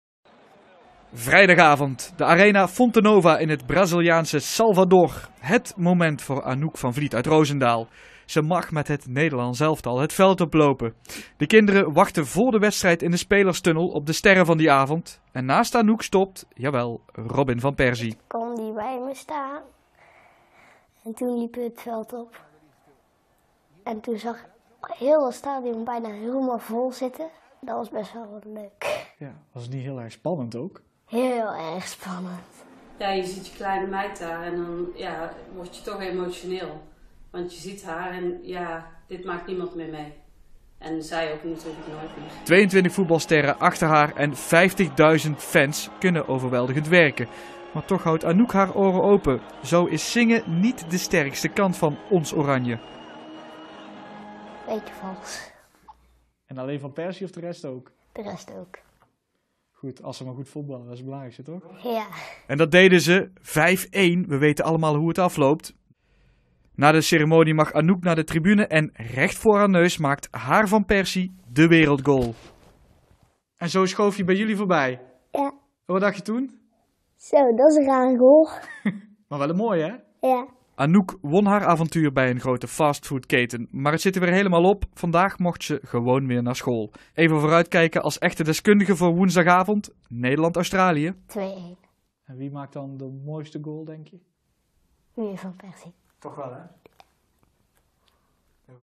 Anouk van Vliet, die met Van Persie het veld op liep, is weer thuis: 'Ze zongen een beetje vals'
De 9-jarige Anouk van Vliet uit Roosendaal mocht vrijdagavond het WK-voetbalveld op met haar held Robin van Persie. Vandaag, terug in Roosendaal, mag ze gewoon weer naar school.  Heel snel zal ze haar het Braziliaanse avontuur niet vergeten, want Anouk mocht hand in hand met Van Persie het duel tegen Spanje openen.